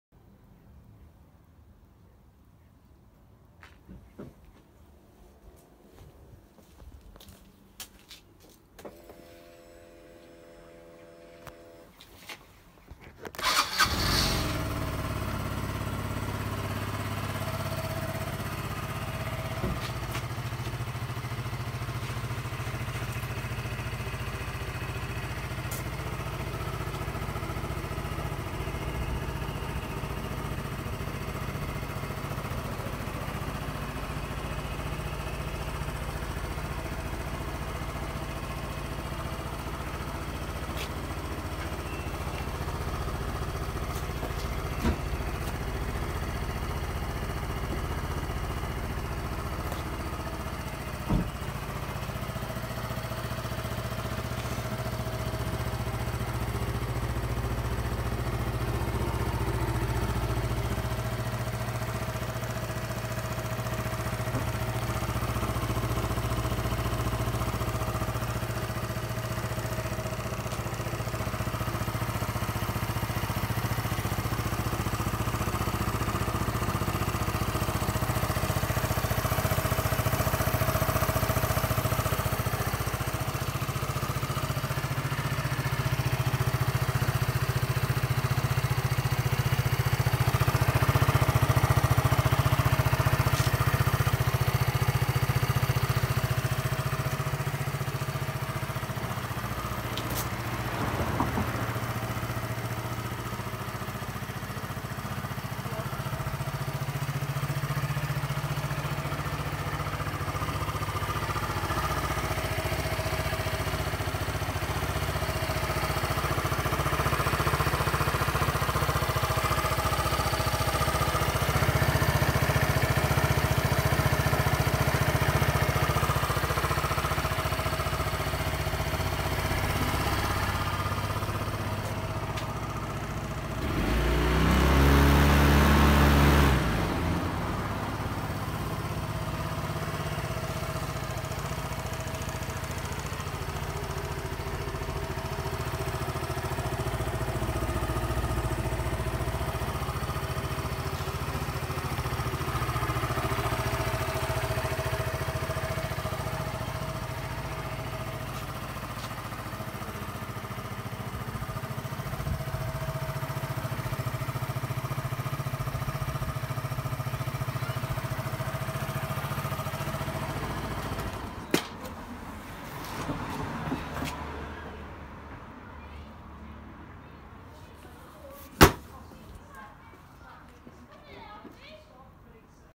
Yamaha Vity 125 Scooter For Sale At Hastings Motorcycle Centre
Here we have a fantastic example of a Yamaha Vity 125. This scooter is in excellent all round condition and has covered just 482 miles from new! There are a couple of very minor marks from road use which I have shown clearly in the video, apart from that this scooter really is clean! These 4 stroke Yamaha scooters are unbeatable for great reliability and low running costs. This scooter benefits from a main and side stand and large under seat storage. This scooter would leave us serviced and with a new M.O.T